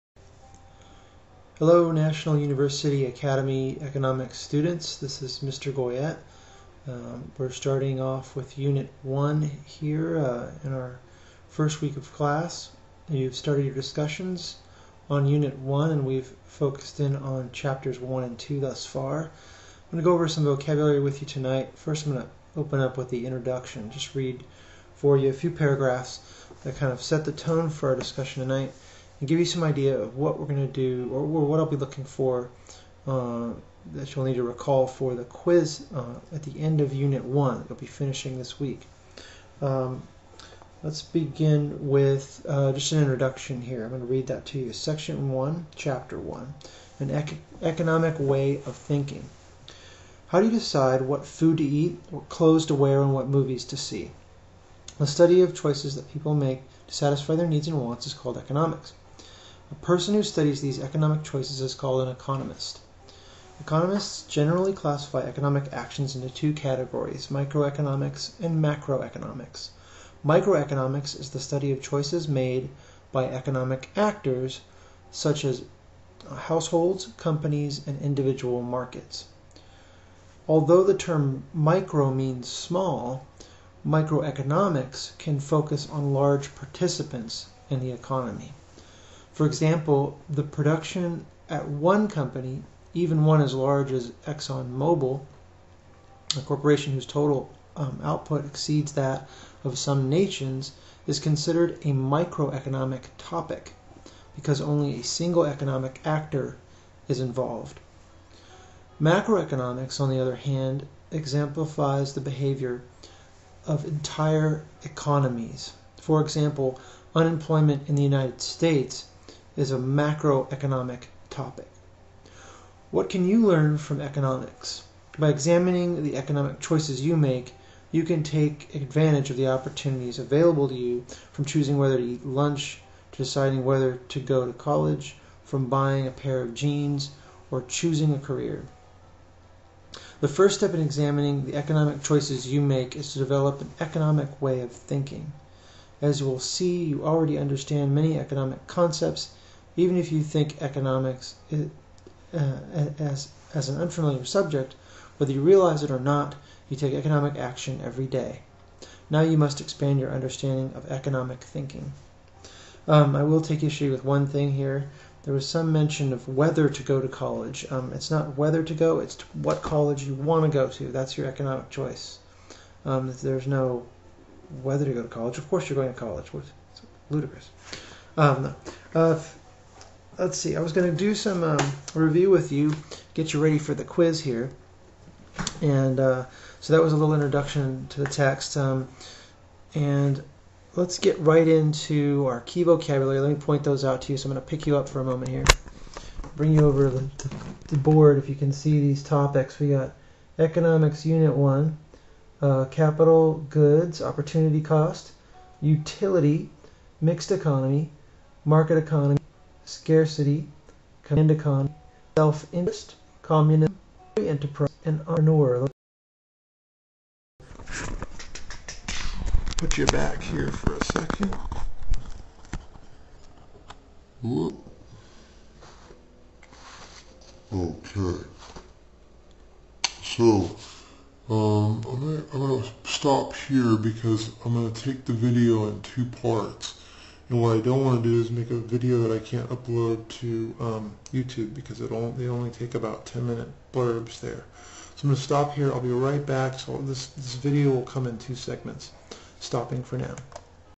Economics: Unit 1 - Intro
Inroduction to economics and a review of the content you will see on th first quiz for unit one. The key terms and topics discussed are: capital goods, opportunity cost, cammand economy, communism, socialism, Adam Smith, self interest, free enterprise and more.